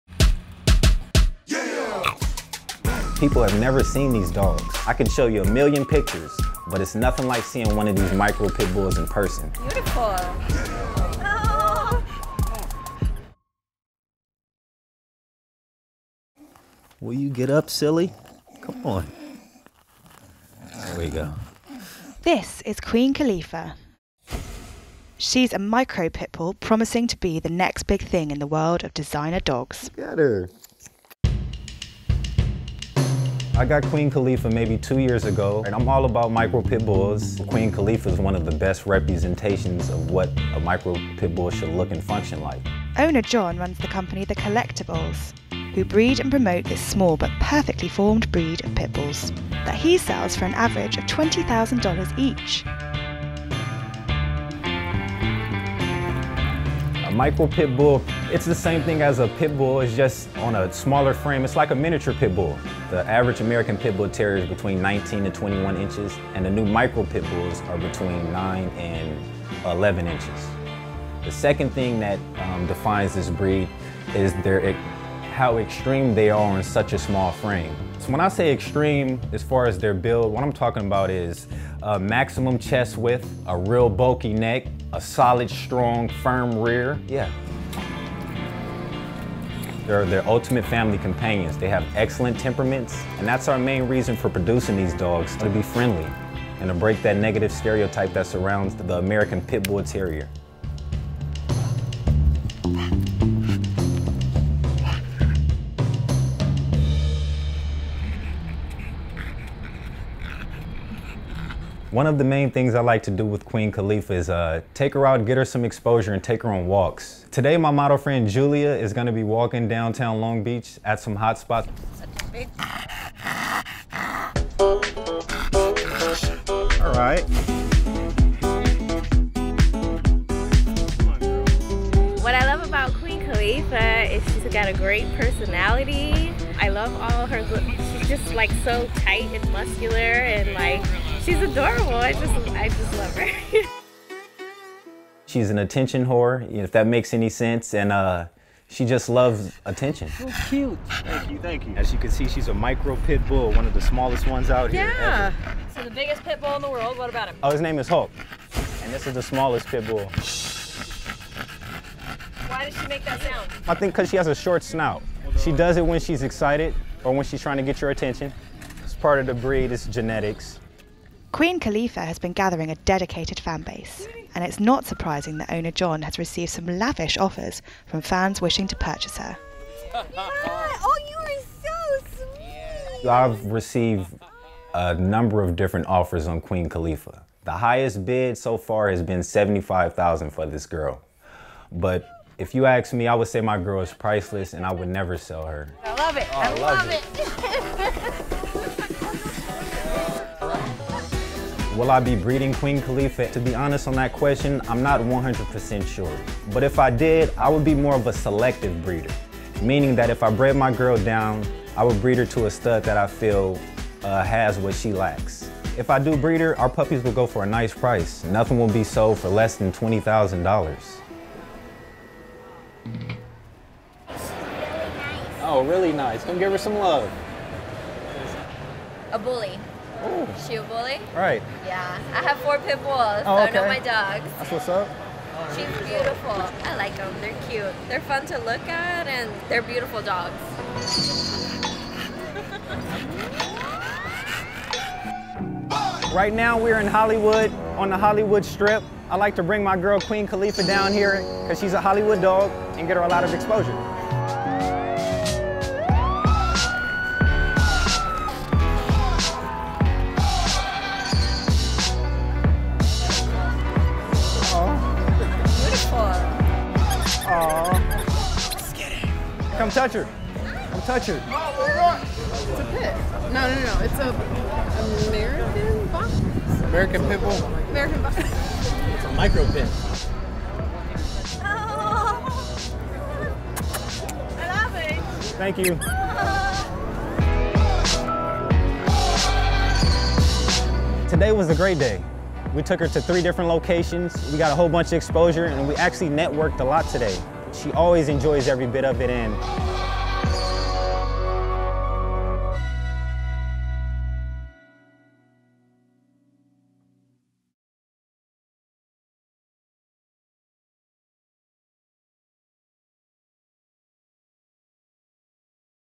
The $75,000 Micro Pit Bull
FORGET Pugs, Pomeranians or Chihuahuas - the rich and famous could soon be carrying a PIT BULL in their Gucci handbags. A new breed of “micro pit bulls” is rapidly becoming the must-have canine companion in the world of designer dogs. John Truitt, 30, from Long Beach in California, runs The Collectibulls, a company which breeds, trains, and sells ‘bullies’ to customers around the world, and is a leading advocate for the breed. His signature dog is Queen Califia, a 12-inch-high “micro pit bull” who has been valued at over $75,000. Regular pygmy pit bulls sell for anywhere from $10,000 to $20,000, with interest coming from as far afield as Italy, China and Australia.

Videographer / director: Joel Forrest
Producer: Tom Midlane, Ruby Coote
Editor: Sonia Estal